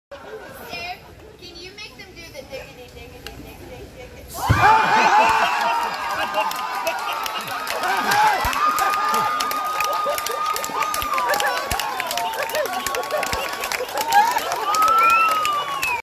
The Washing Well Wenches @ The Georgia Renaissance Festival
The "Washing Well Wenches" having fun with a v̶i̶c̶t̶i̶m̶ volunteer from the audience.